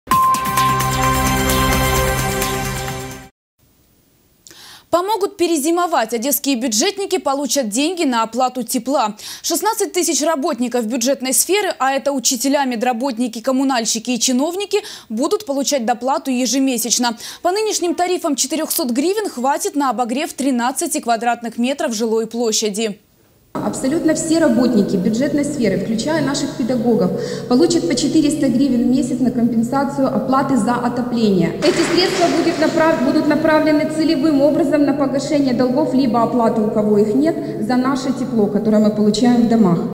Помогут перезимовать. Одесские бюджетники получат деньги на оплату тепла
Шестнадцать тысяч работников бюджетной сферы – а это учителя, медработники, коммунальщики и чиновники – будут получать доплату ежемесячно. По нынешним тарифам четырехсот гривен хватит на обогрев тринадцати квадратных метров жилой площади